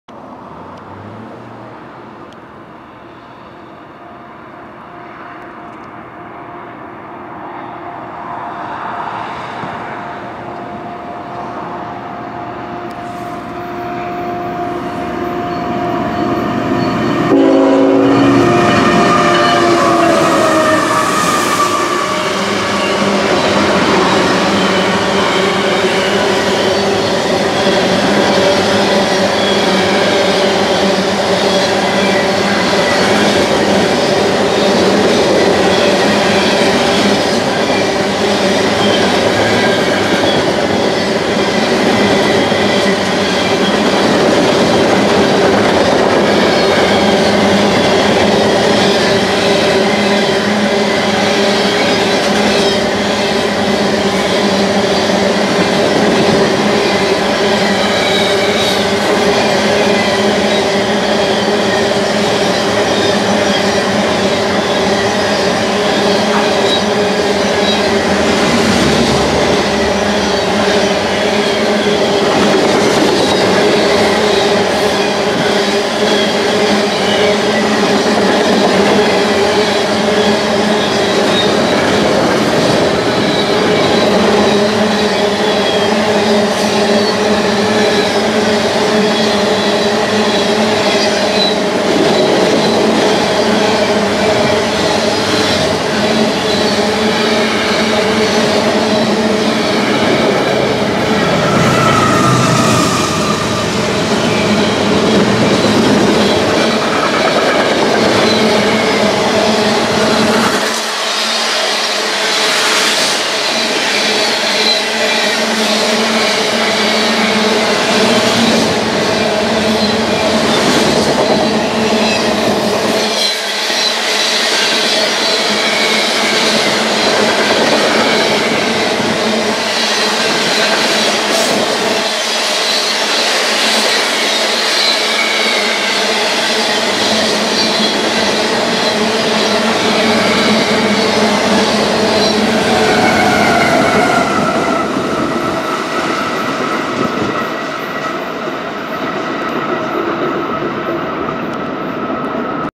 [Horn] - CN B73041-25 - St-Basile Le Grand - (2020-03-25)
CN B73041-25 at St-Basile Le Grand!

Engines: CN 3247 (ET44AC), CN 3012 (ET44AC), Mid DPU CN 3876 (ES44AC TIER 4) and Pusher CN 3002 (ET44AC)!